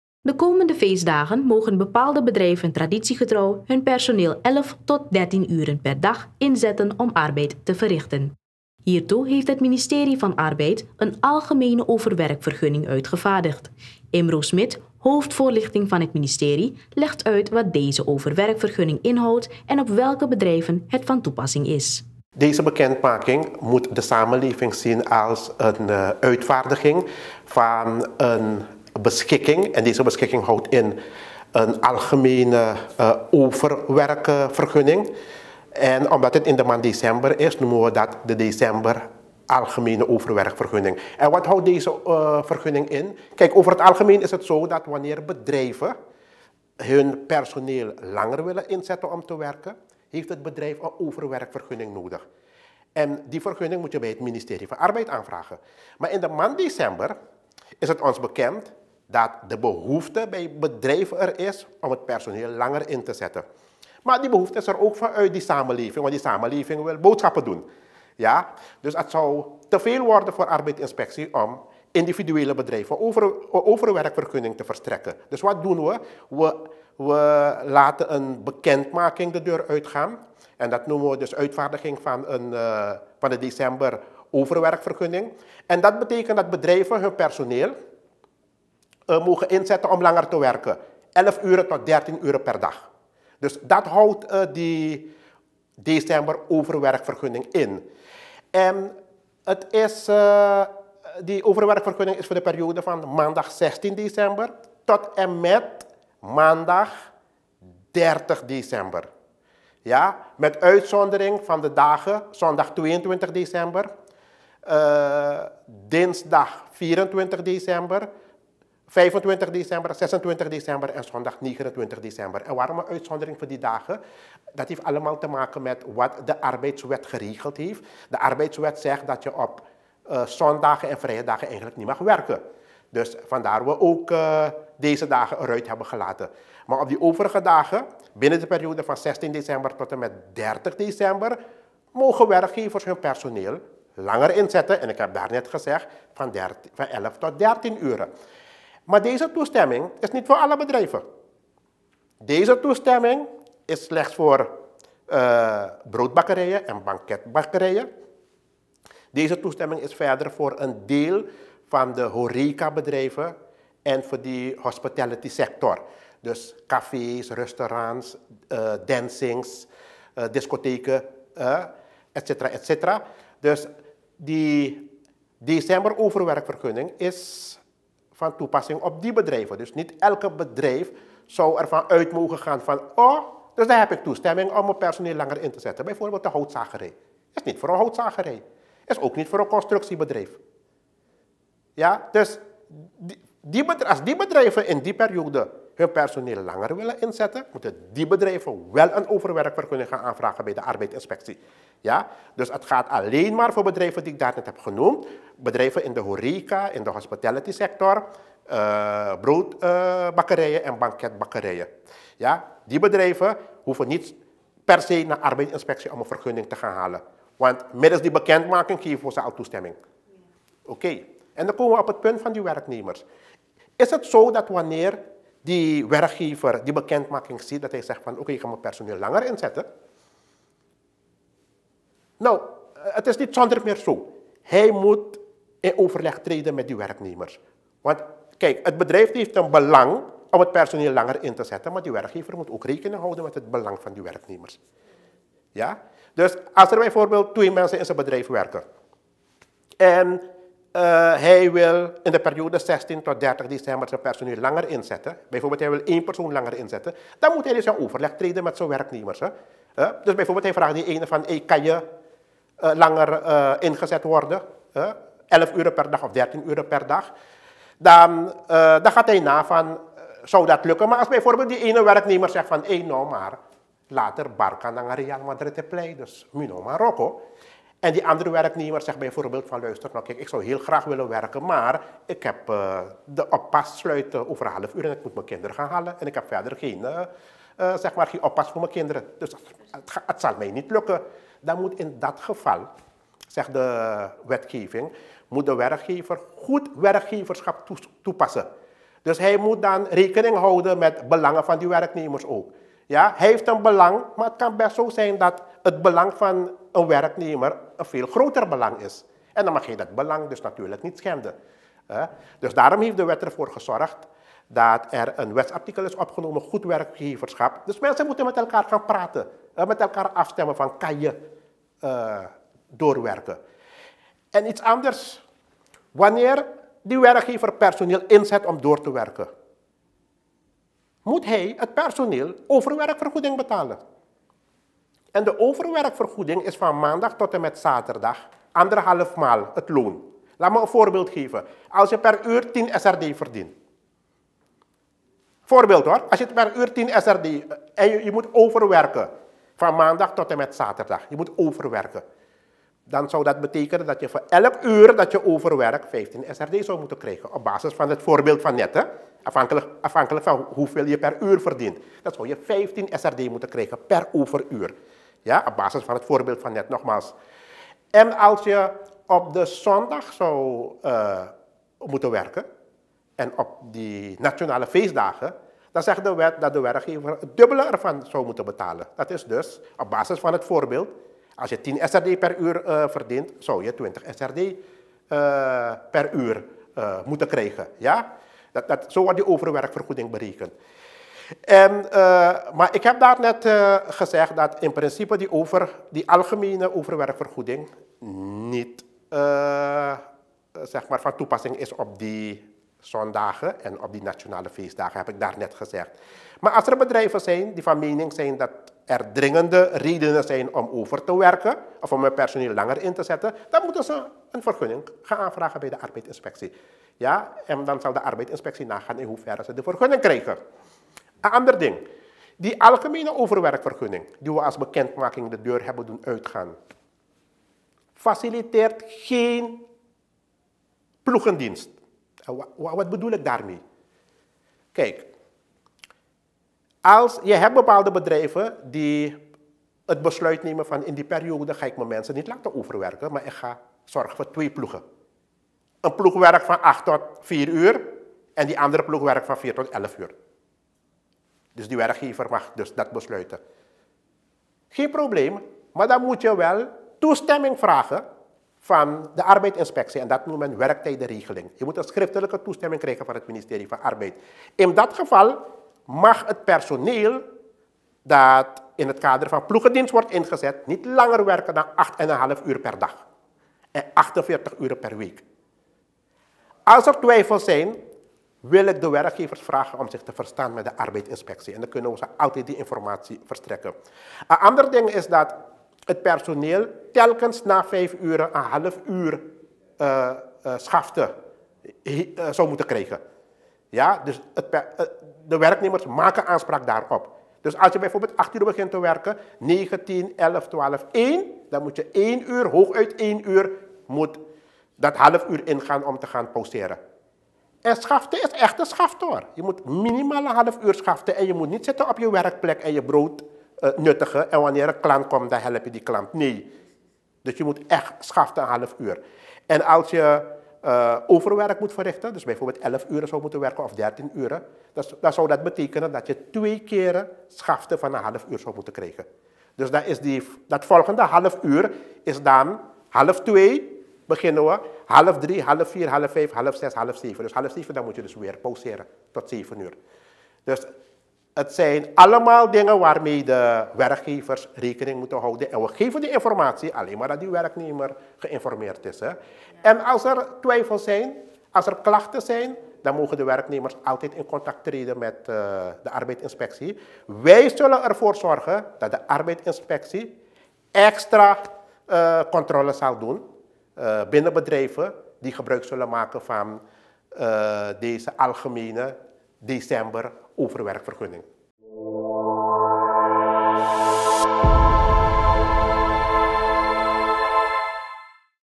MIN. ARBEID VERSCHAFT DUIDELIJKHEID OVERWERKVERGUNNING VOOR FEESTDAGEN
De komende feestdagen mogen bepaalde bedrijven traditiegetrouw hun personeel 11 tot 13 uren per dag inzetten om arbeid te verrichten. Hiertoe heeft het ministerie van Arbeid een Algemene Overwerkvergunning uitgevaardigd. Imro Smith, hoofd Voorlichting van het ministerie, legt uit wat deze Overwerkvergunning inhoudt en op welke bedrijven het van toepassing is.